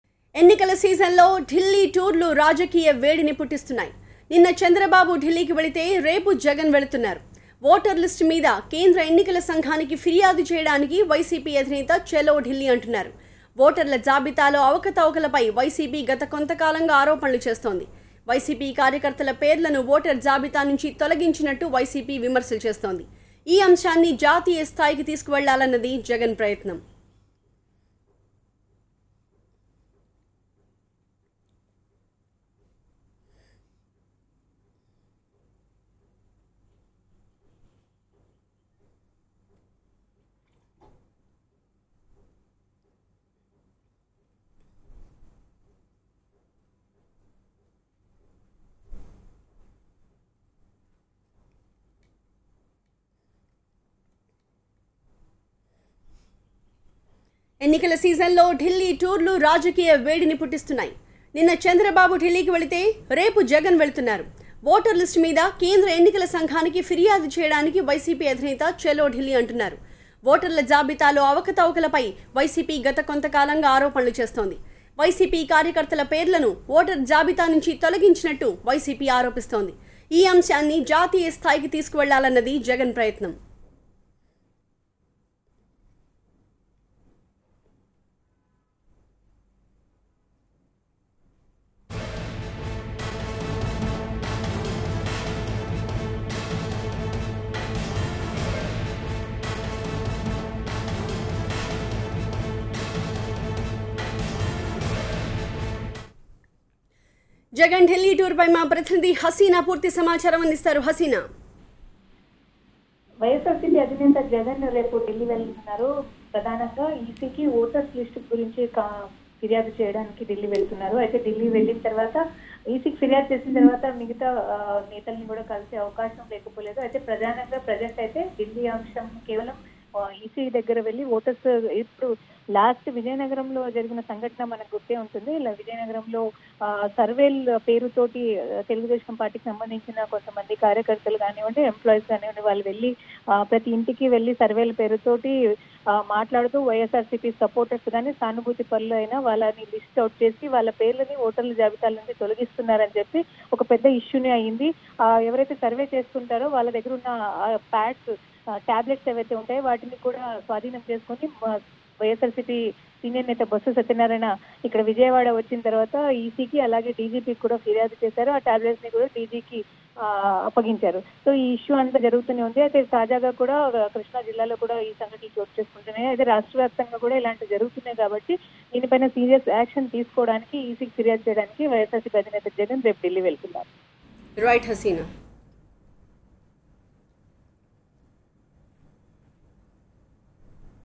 YS Jagan to approach EC tomorrow over EVM issue - TV9
YS Jagan to approach EC tomorrow over EVM issue